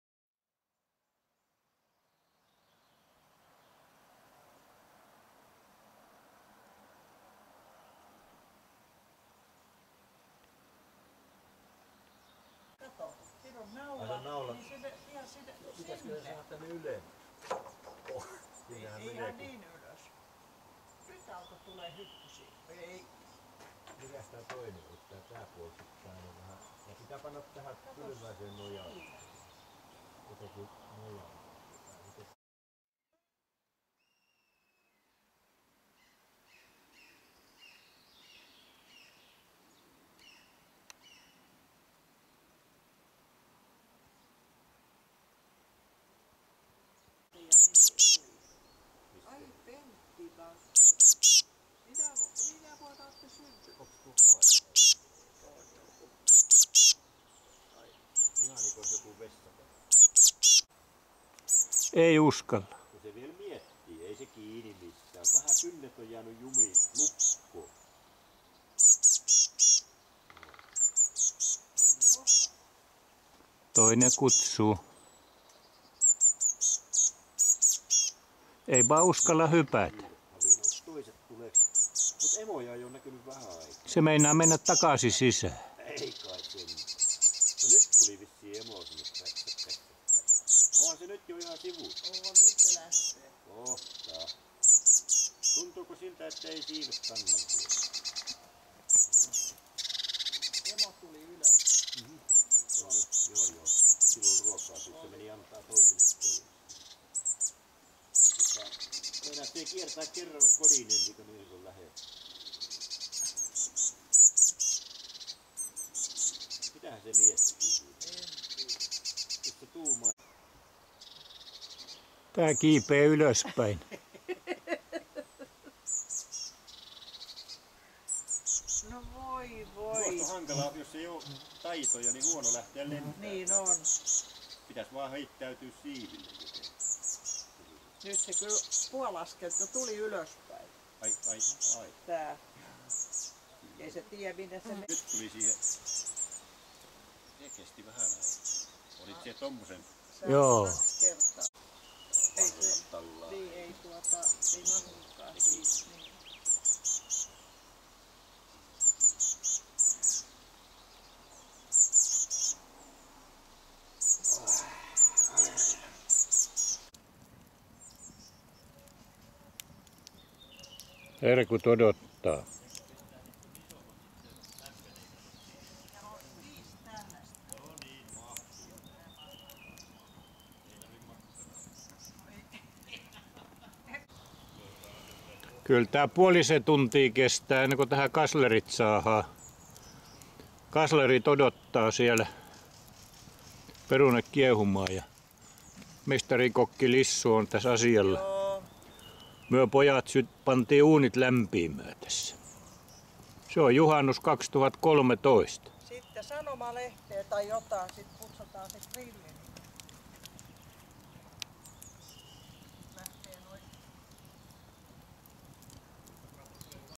NURMIJÄRVI, Juhannusta 2013
Olipa monenmoista juhannusmeininkiä! Tintinpoikanen puunrungolla Kurkkii reiästä, tulee ulos. emo